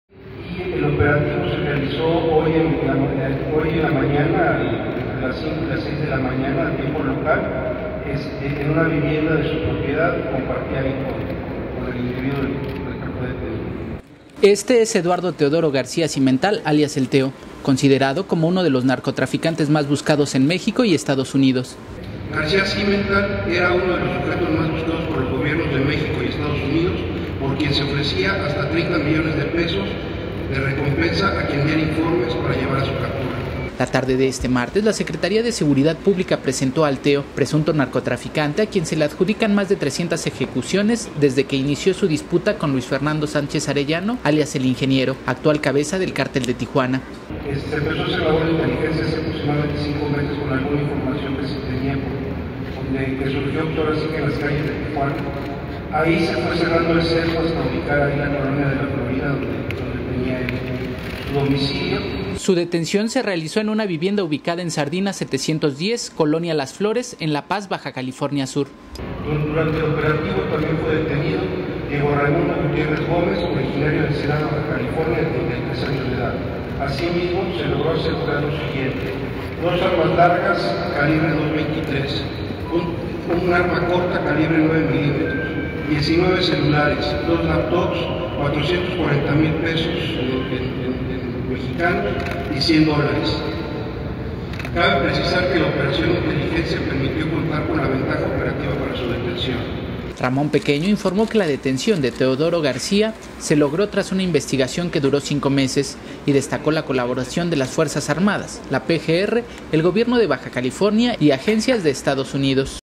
Presenta SSP a 'El Teo'
La SSP presentó a Teodoro García, 'El Teo', quien fue detenido en La Paz, BCS; se le atribuyen más de 300 ejecuciones con sanguinarios métodos.